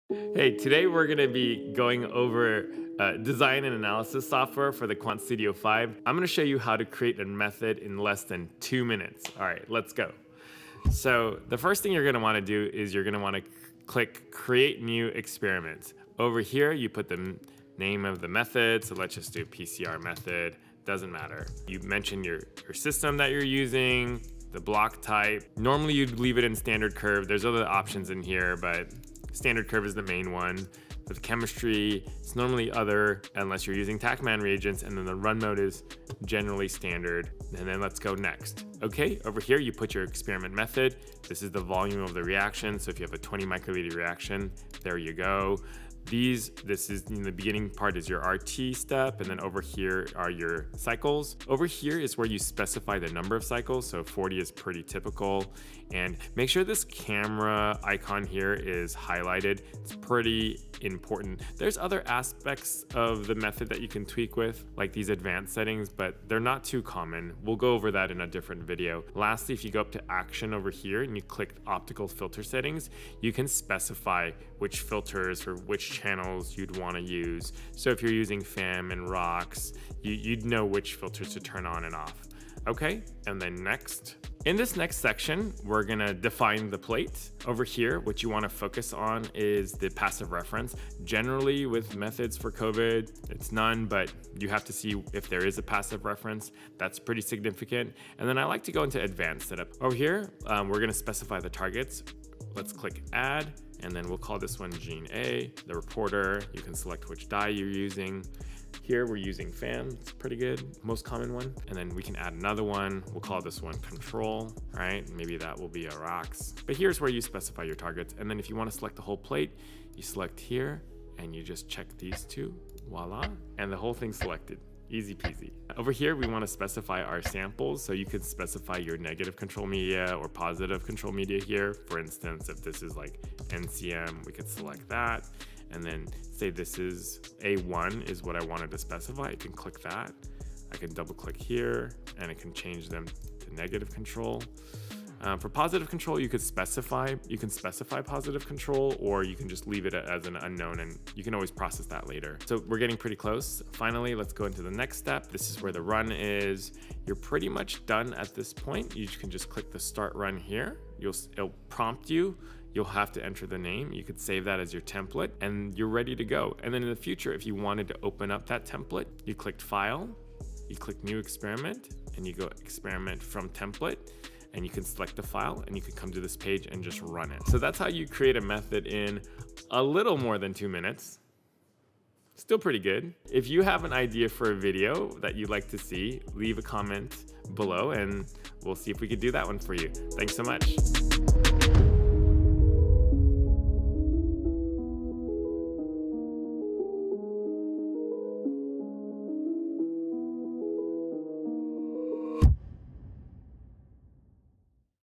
How to Create a Method in LESS Than 2 MINUTES in Design and Analysis for the QuantStudio 5
Want to learn a fast way to create your method in Design and Analysis for the QuantStudio 5? Boom, I got you.

We will go over how to set up your method quickly so you can start testing and save time stumbling through DA to create your method.

⬇️ Get Affordable and Dope Lab Consumables Here ⬇️
(No pun intended, unless you're a cannabis lab, then pun intended)